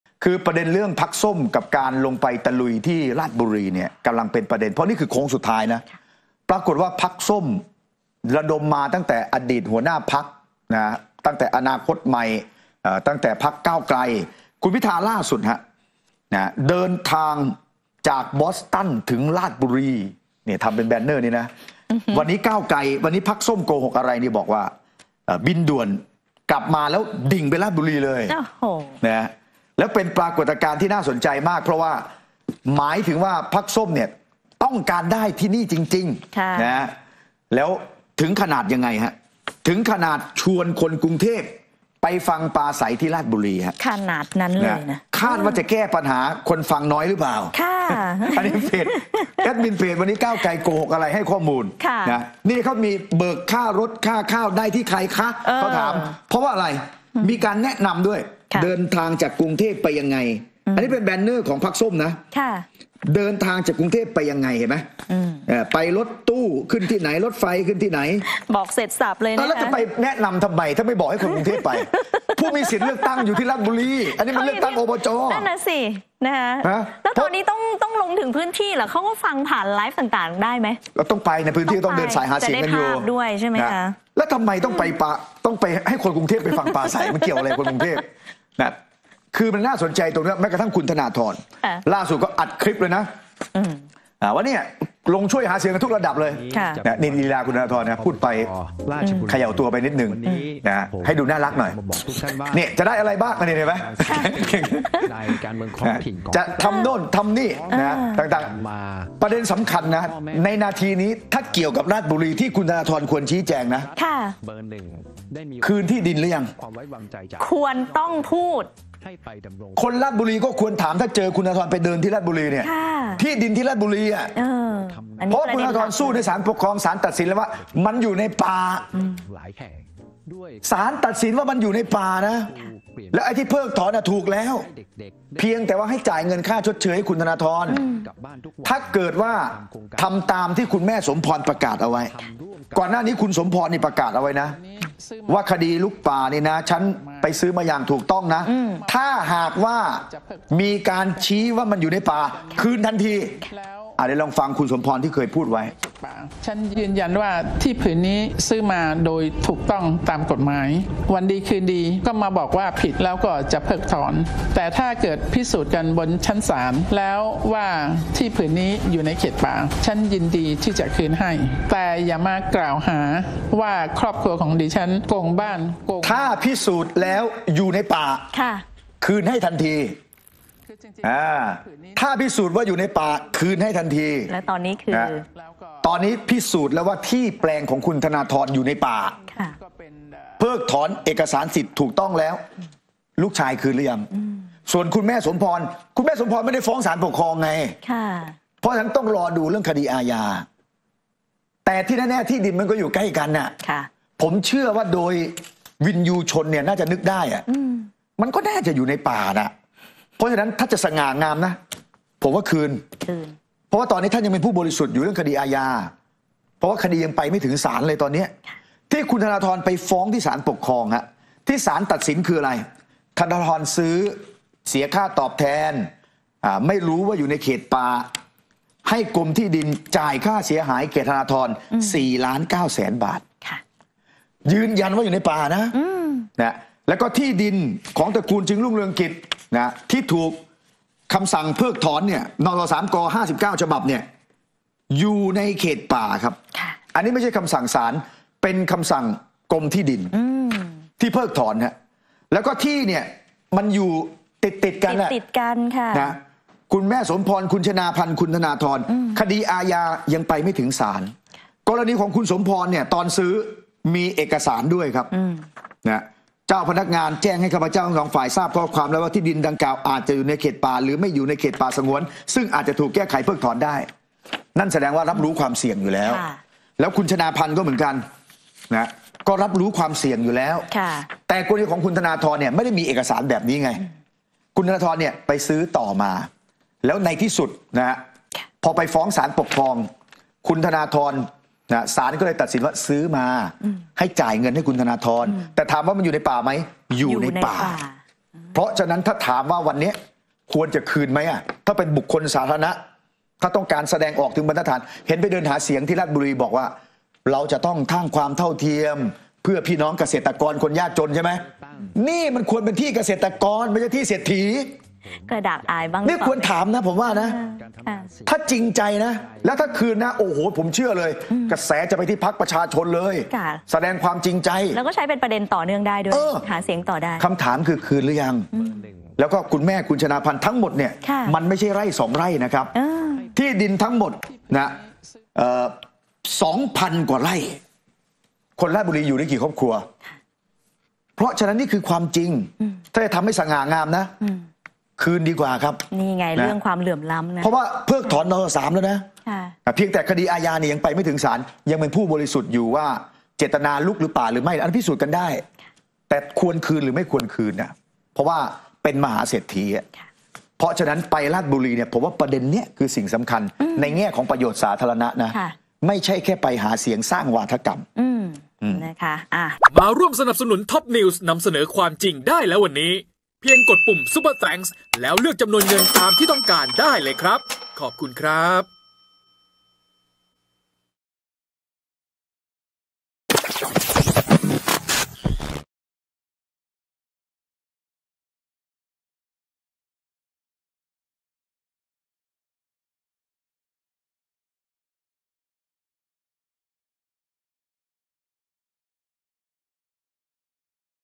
"พรรคส้ม" กลัวขายหน้า "พิธา" บินด่วนบอสตันดิ่งตรงราชบุรี ระดมพลคนกทม.ร่วมฟังปราศรัย
"พรรคส้ม" กลัวขายขี้หน้า "พิธา" บินด่วนบอสตันดิ่งตรงราชบุรี ระดมพลคนกทม.ร่วมฟังปราศรัยเลือกตั้งท้องถิ่น "สันติสุข" ปลุกชาวบ้านทวงคืนที่ผืนป่าคืนจาก "ธนาธร"